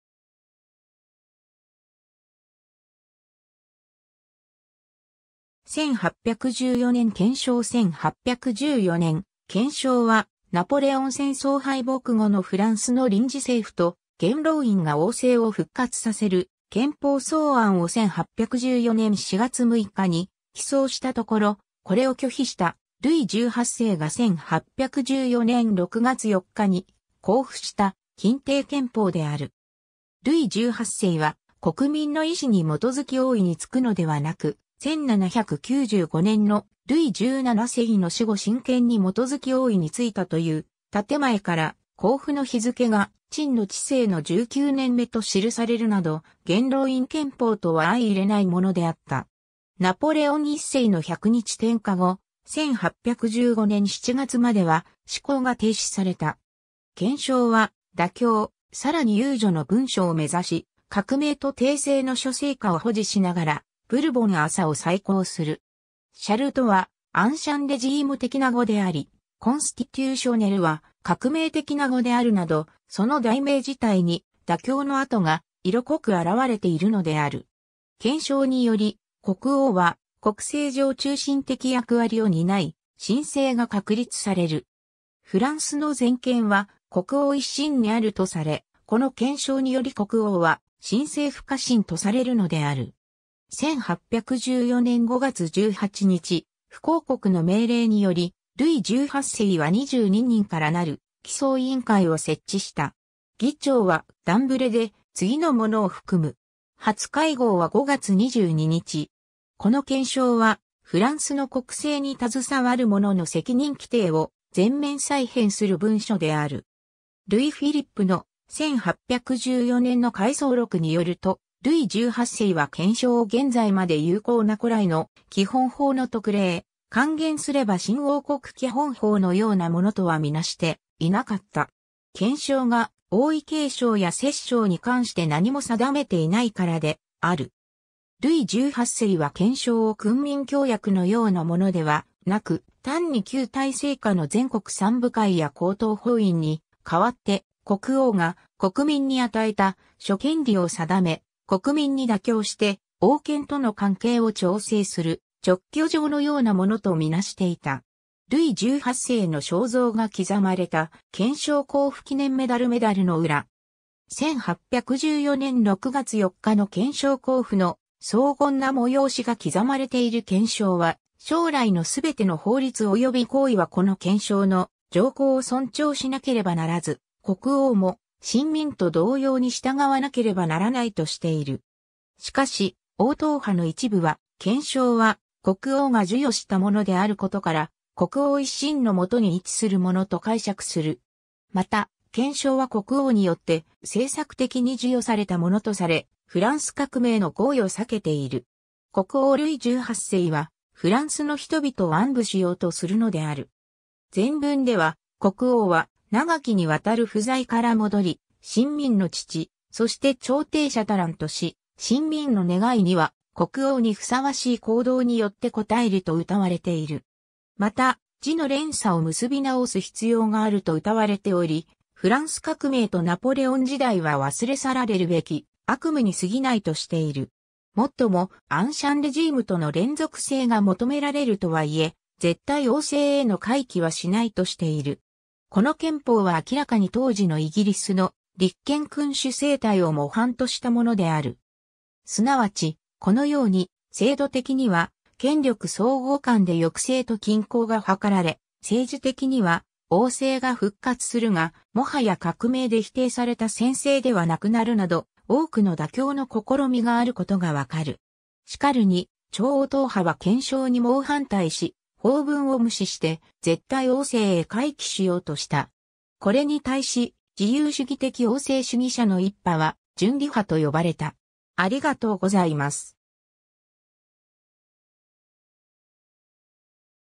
1814年憲章
1814年憲章 1814年憲章（フランス語: Charte constitutionnelle de 1814）は、ナポレオン戦争敗北後のフランスの臨時政府と元老院が王政を復活させる憲法草案（元老院憲法）を1814年4月6日に起草したところ、これを拒否したルイ18世が1814年6月4日に公布した欽定憲法である。
 ルイ18世（当時のプロヴァンス伯爵ルイ・スタニスラス・グザヴィエ）は、国民の意志に基づき王位に就くのではなく、1795年のルイ17世の死後神権に基づき王位に就いたという建前から、公布の日付が「朕の治世の19年目（notre règne le dix-neuvième）」と記されるなど、元老院憲法とは相容れないものであった。
ナポレオン1世の百日天下後、1815年7月までは施行が停止された。
 憲章は妥協、さらに宥恕の文書を目指し、革命と帝政の諸成果を保持しながらブルボン朝を再興する。
「シャルト（Charte）」はアンシャン・レジーム的な語であり、「コンスティテューショネル（constitutionnelle）」は革命的な語であるなど、その題名自体に妥協の跡が色濃く表れているのである。
 憲章により、国王は国政上中心的役割を担い、親政が確立される。
「フランスの全権は国王一身にある（L'autorité tout entière (réside) en France dans la personne du Roi）」とされ、この憲章により国王は「神聖不可侵（inviolable et sacrée）」とされるのである。
 1814年5月18日、不公告の命令により、ルイ18世は22人からなる「起草委員会（commission de rédaction）」を設置した。
 議長はダンブレで、次の者を含む。
 初会合は5月22日。
 この憲章は、フランスの国政に携わる者（国王と両議院）の責任規定を全面再編する文書である。
 ルイ＝フィリップの「1814年の回想録（Souvenirs de 1814）」によると、ルイ18世は憲章を現在まで有効な古来の基本法の特例、換言すれば新王国基本法のようなものとはみなしていなかった。
憲章が王位継承や摂政に関して何も定めていないからである。
ルイ18世は憲章を君民協約のようなものではなく、単に旧体制下の全国三部会や高等法院に代わって、国王が国民に与えた諸権利を定め、国民に妥協して王権との関係を調整する勅許状のようなものとみなしていた。
 ルイ18世の肖像が刻まれた憲章公布記念メダル メダルの裏。
1814年6月4日の憲章公布の荘厳な催しが刻まれている 憲章は、将来のすべての法律および行為はこの憲章の条項を尊重しなければならず、国王も臣民と同様に従わなければならないとしている。
しかし、王党派の一部は、憲章は国王が授与したものであることから、国王一身の下に位置するものと解釈する。
 また、憲章は国王によって政策的に授与されたものとされ、フランス革命の語彙を避けている。
国王ルイ18世はフランスの人々を安撫しようとするのである。
 前文では、国王は「長きにわたる不在（longue absence）」から戻り、臣民の父、そして調停者たらんとし、「臣民の願い（vœu du peuple）」には「国王にふさわしい（digne du roi）」行動によって応えるとうたわれている。
 また、「時の連鎖を結び直す（renouer la chaîne des temps）」必要があるとうたわれており、フランス革命とナポレオン時代は忘れ去られるべき悪夢にすぎないとしている。
もっとも、アンシャン・レジームとの連続性が求められるとはいえ、絶対王政への回帰はしないとしている。
 この憲法は明らかに当時のイギリスの立憲君主政体を模範としたものである。
すなわち、 このように、制度的には権力相互間で抑制と均衡が図られ、政治的には王政が復活するが、もはや革命で否定された専制ではなくなるなど、多くの妥協の試みがあることがわかる。
 しかるに、超王党派は憲章に猛反対し、法文を無視して絶対王政へ回帰しようとした。
これに対し、自由主義的王政主義者の一派は純理派と呼ばれた。